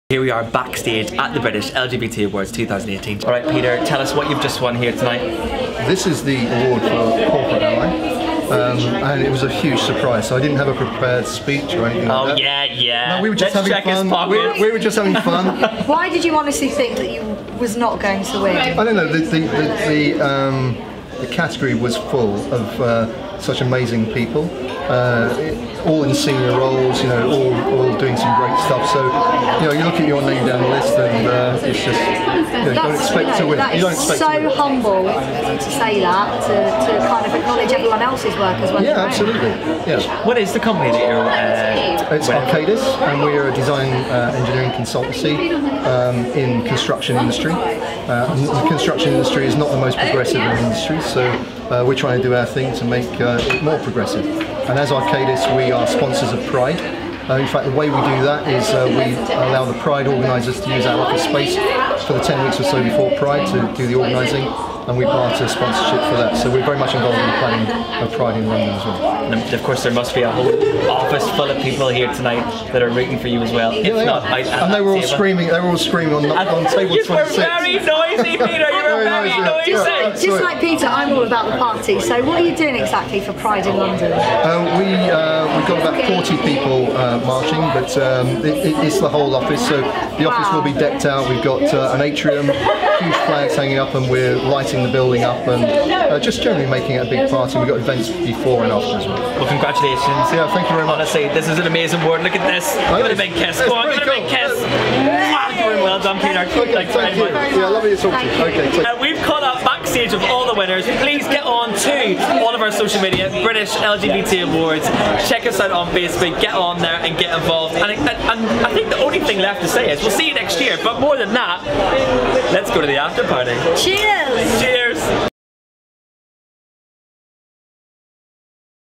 Backstage With Corporate Ally Winner Peter Madden | British LGBT Awards 2018
So, the British LGBT Awards might be over for another year... but we have some behind the scenes extras to keep you entertained a while longer. Here is Peter Madden, winner of Corporate Ally.